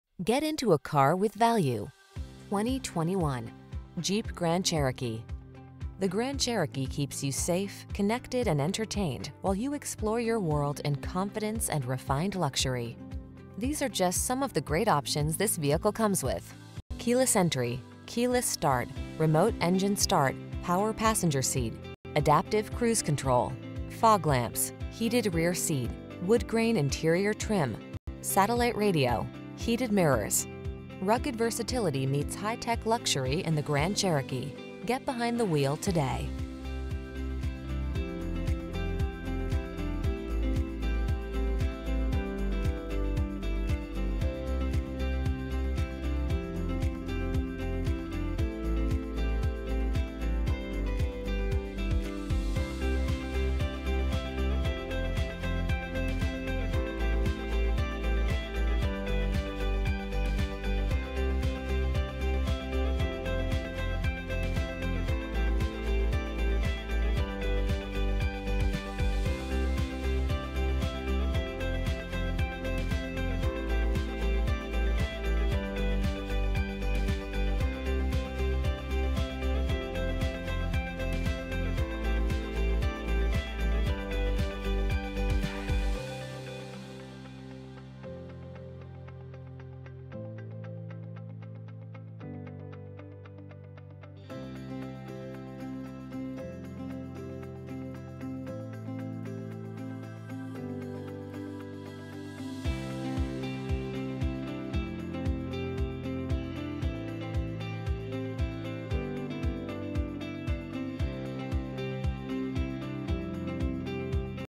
2021 Jeep Grand Cherokee L New Smyrna Beach, Port Orange, Daytona Beach, Deltona, Sanford, FL 819200
Bright White Clearcoat New 2021 Jeep Grand Cherokee L available in New Smyrna Beach, Florida at New Smyrna Chrysler. Servicing the Port Orange, Daytona Beach, Deltona, Sanford, FL area.
2021 Jeep Grand Cherokee L Limited - Stock#: 8192009 - VIN#: 1C4RJJBG5M8192009